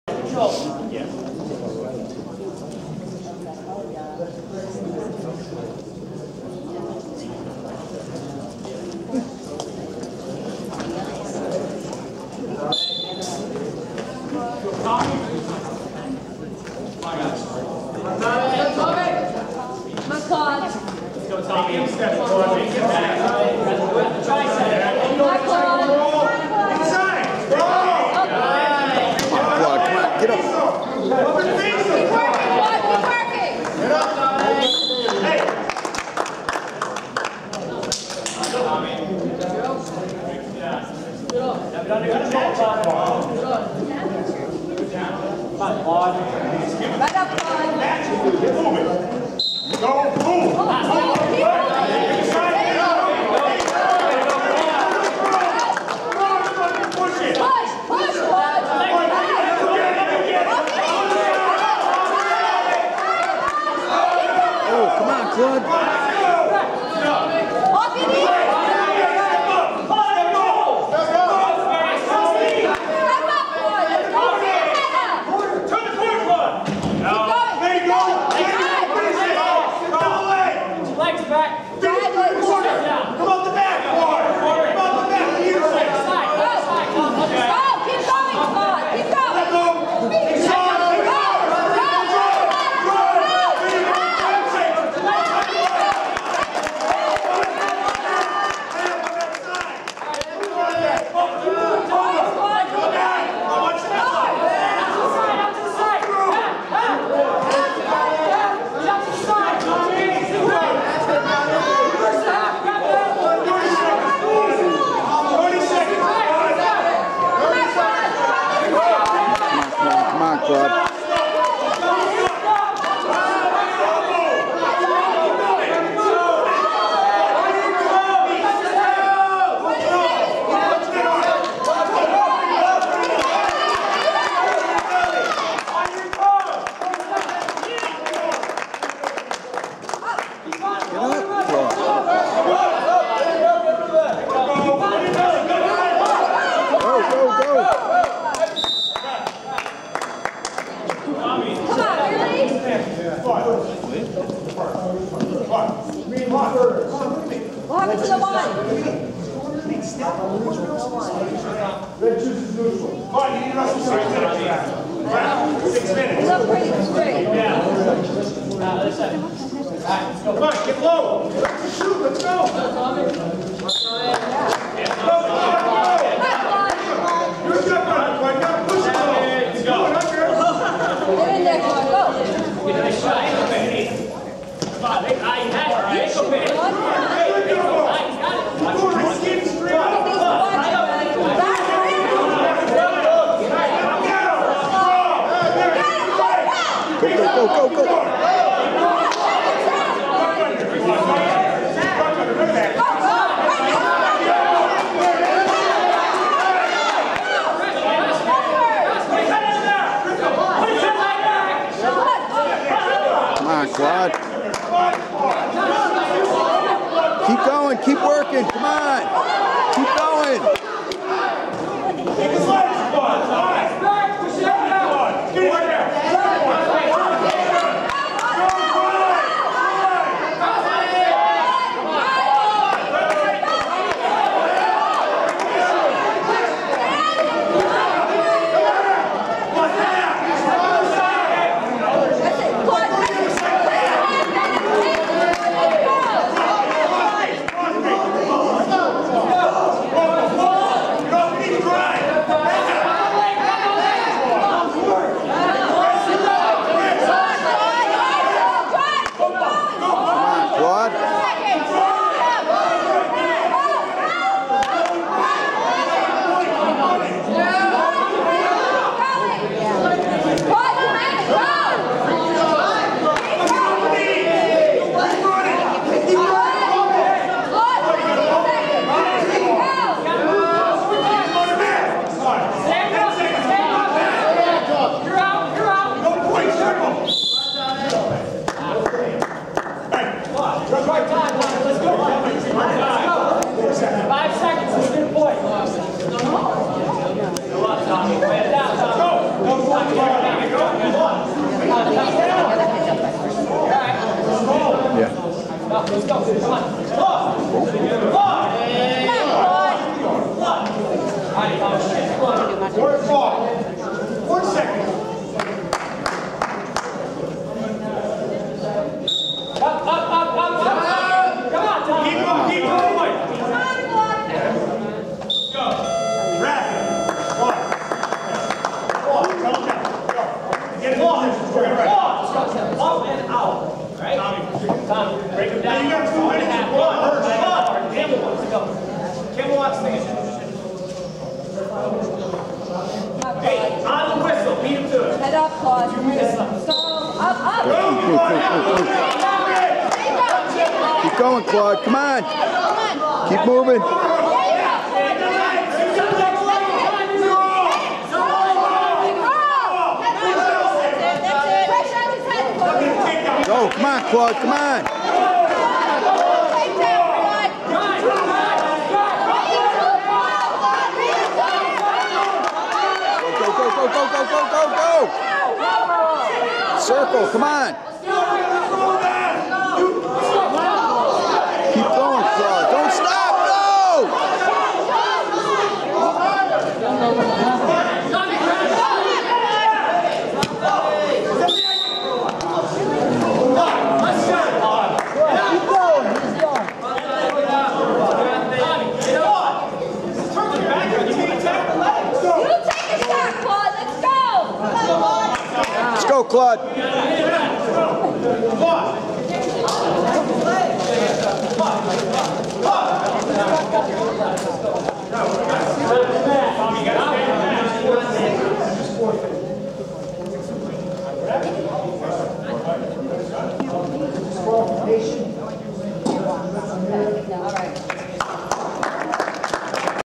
WoodHall 160 ClaudeR Chase Collegiate Wrestling vs WoodHall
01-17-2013 - Chase Collegiate Wrestling - ClaudeR 160 lbs vs Woodhall